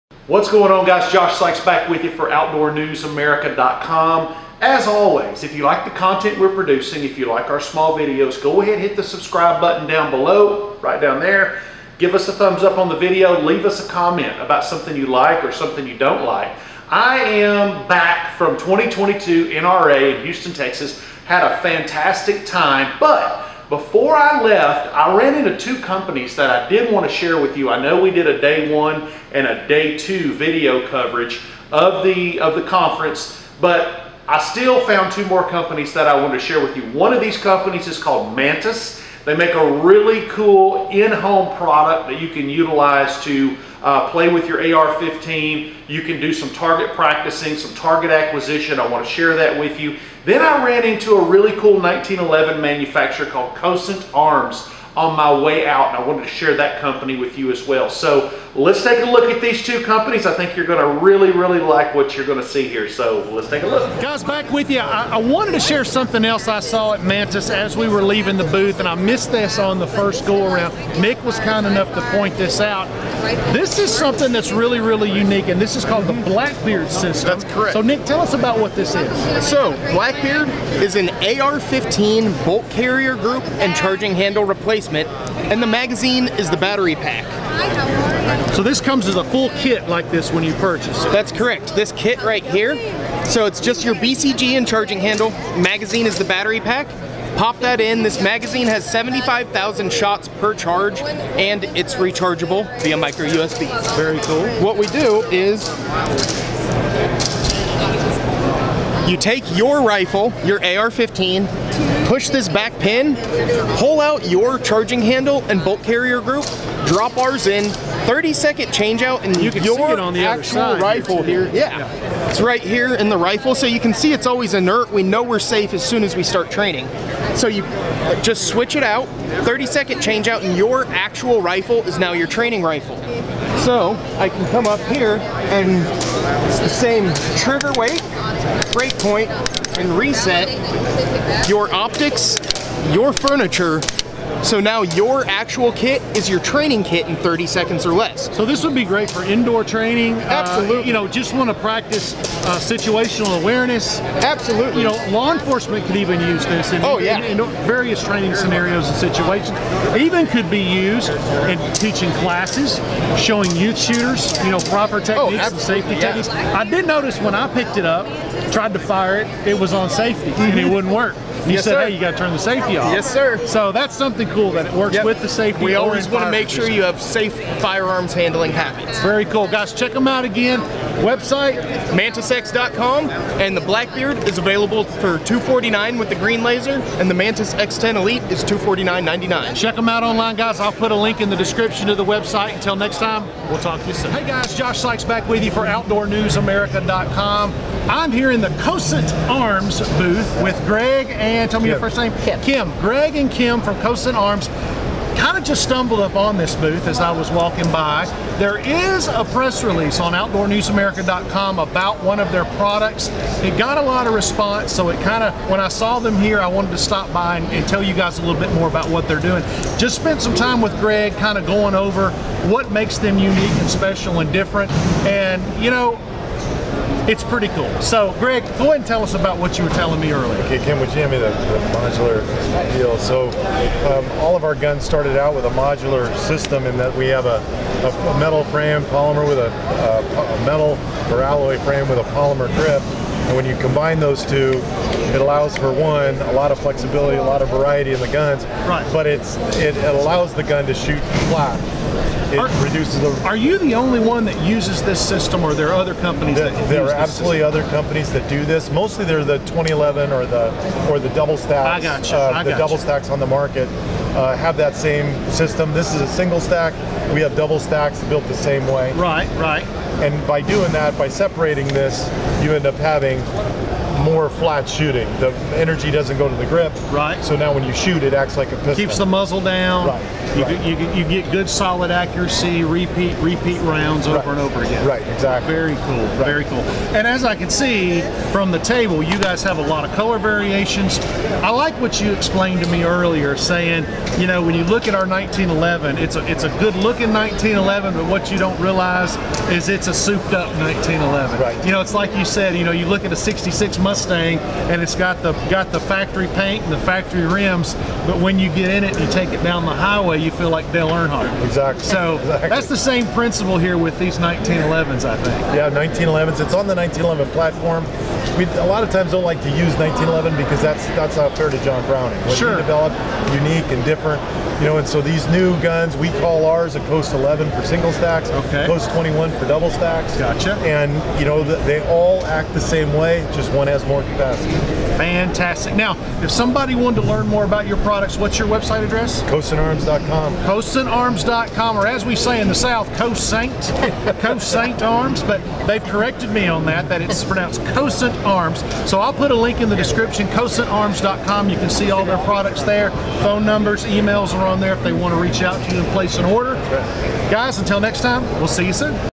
NRA Show 2022 Additional Coverage
We had two more companies we wanted to share with you. Mantis and Cosaint Arms. Both were kind enough to talk with us late in the day so we wanted to make sure we worked them into our coverage. are their web sites!